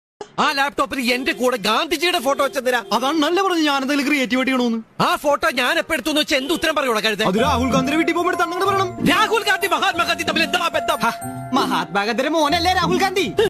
മഹാത്മാ ഗാന്ധിയുടെ മോനല്ലേ രാഹുൽ ഗാന്ധി? - Nandana Chandran. Sreekandapuram, GHSS Nedungome
മഹാത്മാ ഗാന്ധിയുടെ മോനല്ലേ രാഹുൽ ഗാന്ധി? - Nandana Chandran, Icheri, Sreekandapuram, GHSS Nedungome

Symphony Music ലെ ഗായികയും ചങ്ങാതിക്കൂട്ടം ആർട്സിലെ മൾട്ടി ടാലെന്റ്റ് ആർട്ടിസ്റ്റും Nedungome GHSS ലെ വിദ്യാർത്ഥിയും ശ്രീകണ്ഠപുരം ഐച്ചേരി സ്വദേശിനിയുമായ നന്ദന ചന്ദ്രൻ അവതരിപ്പിച്ച Comedy YouTube Shorts, Short Video